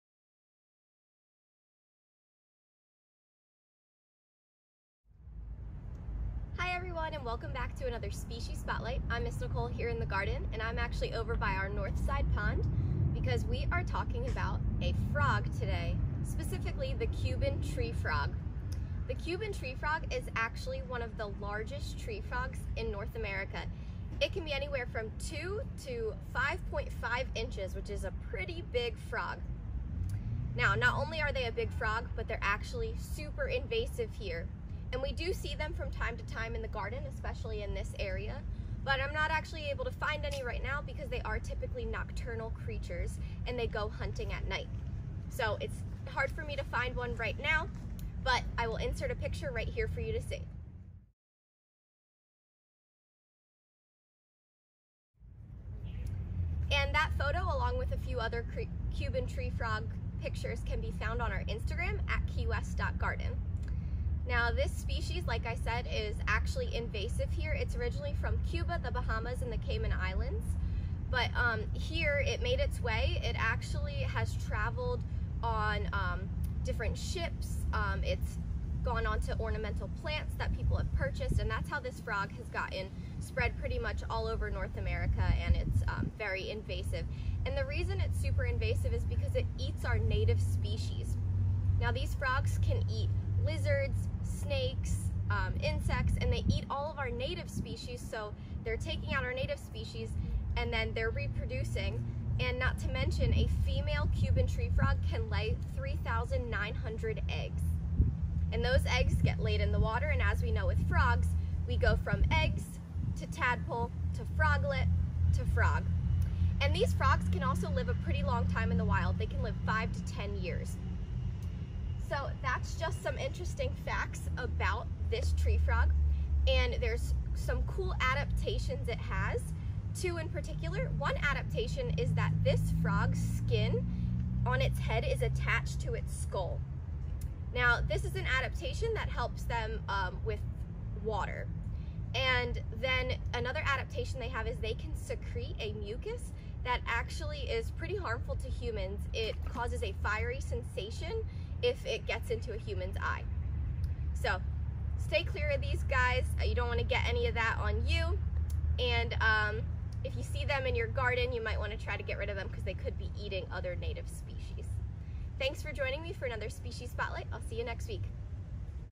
Species Spotlight: Cuban Tree Frog
Did you know Cuban Tree Frogs are the largest tree frogs found in North America?!

To learn more about this invasive species check out our Virtual Living Lab by registering for free at Keywest.garden/education